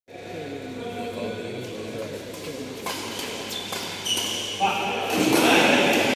007 James & Mahi playing against Pilot Balaji & Tarun in 007 badminton திருவிழா 47 th Event
Video from 007 Airtel